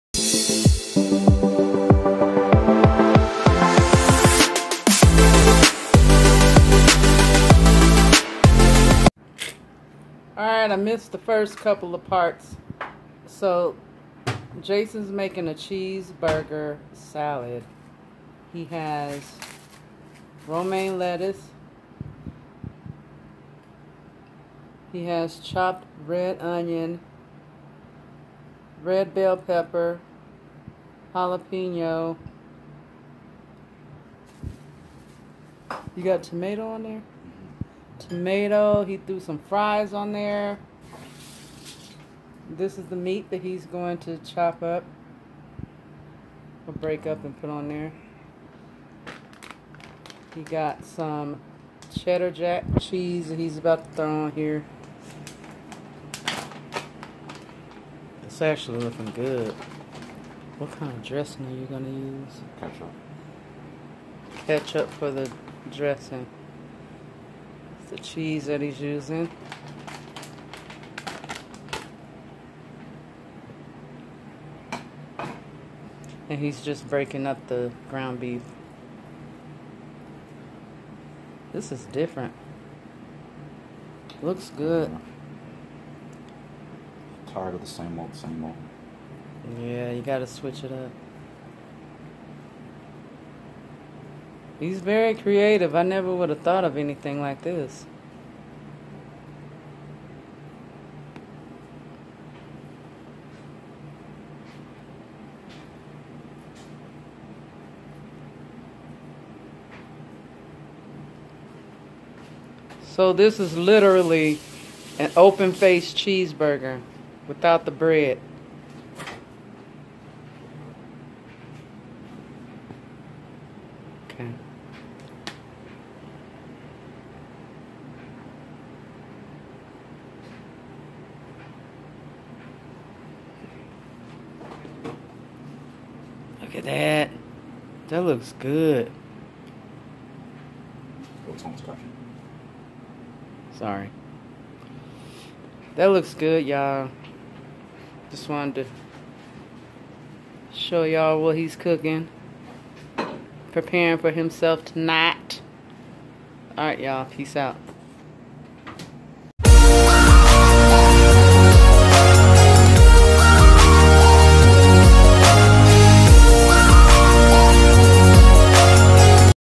Cheeseburger Salad | Another Jason Creation
Heyyyy Playmates!!!

Ingredients for this delicious salad

Romaine lettuce
Tomatoes
Jalapenos
Green Bell Peppers
Red Onion
Cheddar Jack cheese
Ground Beef
French Fries
Ketchup

You seen how he layered it...there is no specific way of layering

This came out looking great!!

Tha is got watching fam!!!